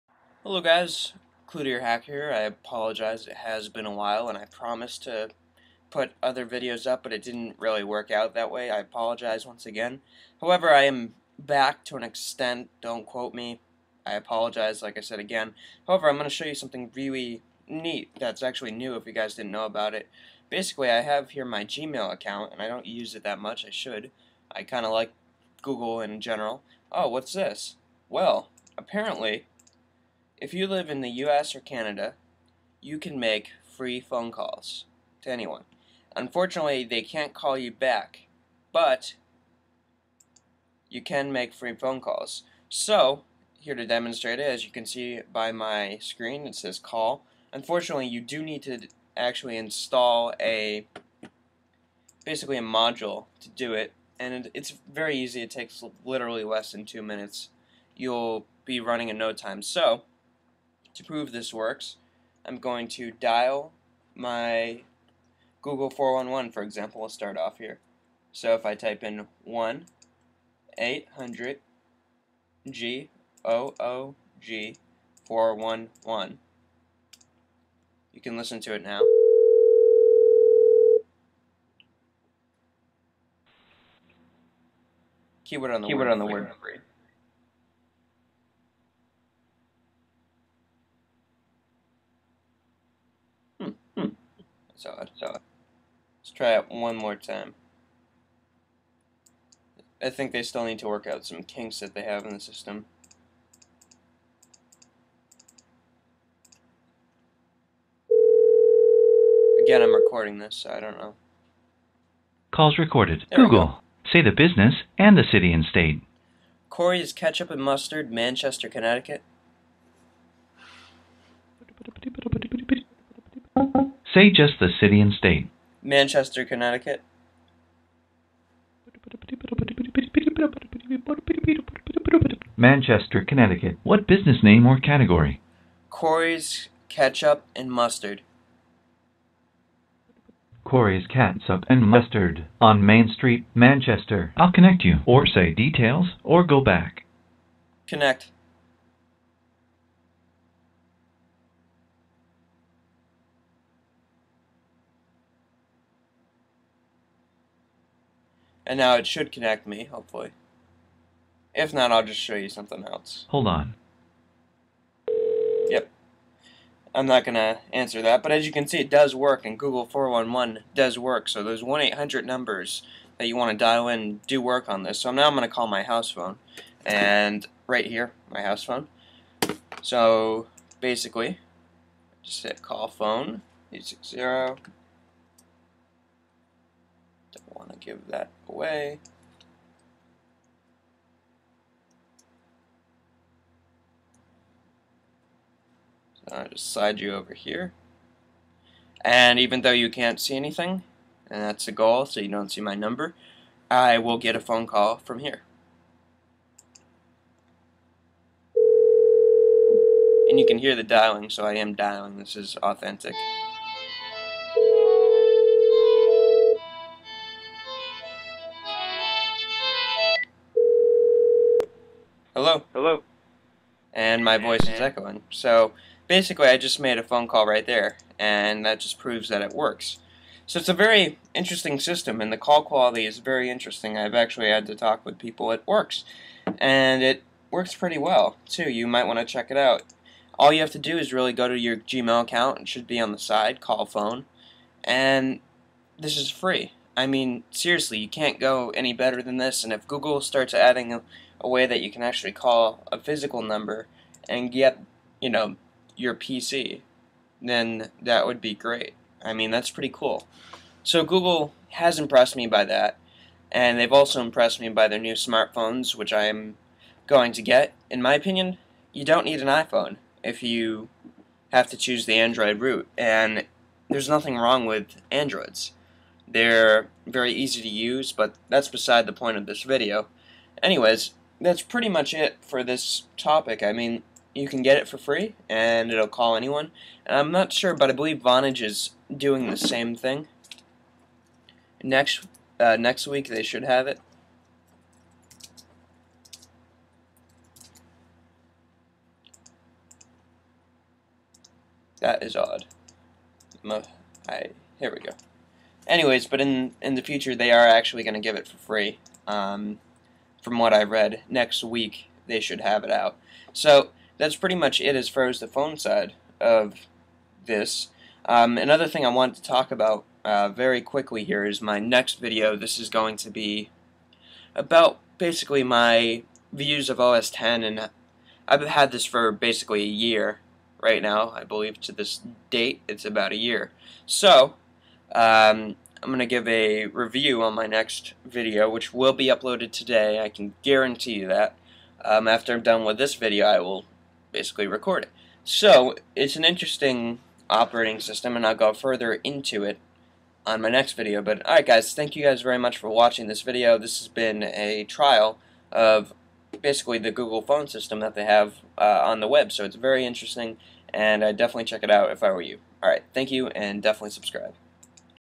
New Google Phone (Free phone calls to anyone in US & Canada)
Hey Guy's Cloutierhaker here, today I am going to show you google's newest feature of gmail. This is a very intersting addon that lets you make free phone calls to any one in the US& Canada. They also offer international calling for 2 cents a minut.